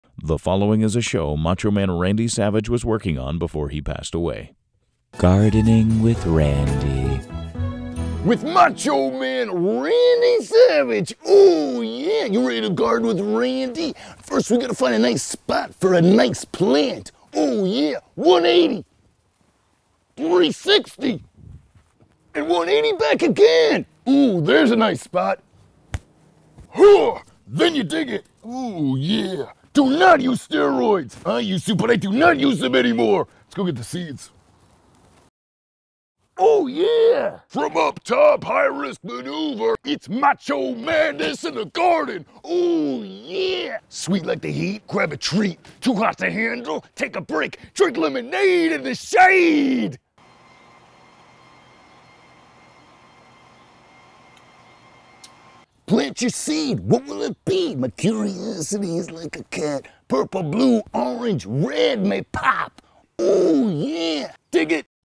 Macho Man Randy Savage Gardening Show
Before "Macho Man" Randy Savage passed away, he was working on a new show for HGTV.

*Impression performed by Adam Hamway